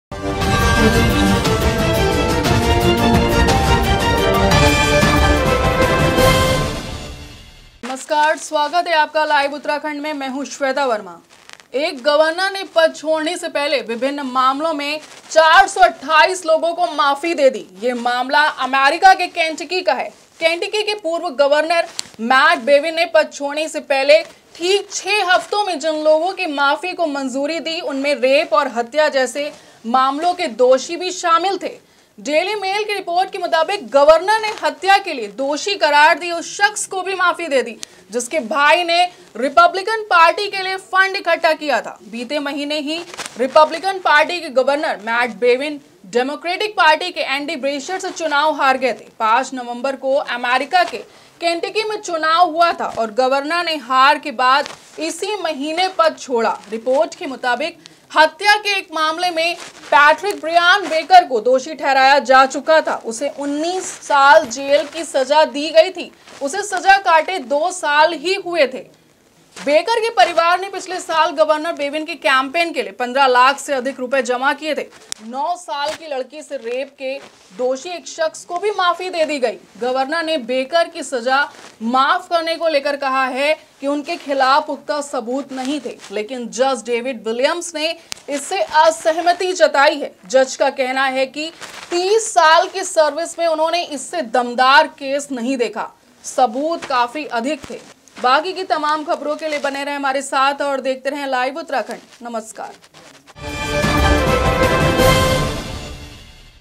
Shamefull : 428 Rapists और Killers को अपने इस्तीफे से पहले इस गवर्नर ने कर दिया माफ | Matt Bevin
Former Kentucky Gov. Matt Bevin issued hundreds of pardons and commutations of sentences in his final days in office after his November defeat to Democrat Andy Beshear, some of which are for violent offenses.

According to the secretary of state's website, as many as 161 pardons and 419 commutations of sentences were filed on December 11, including commutations for 336 people who were serving sentences solely for drug related charges.

When a governor signs an executive order, it goes into effect at that time, the secretary of state's office said.

The Kentucky secretary of state's office began filing the majority of these executive orders into their public system on December 11, their website shows.

The office says due to the high volume of final actions by Bevin, the documents are still being processed.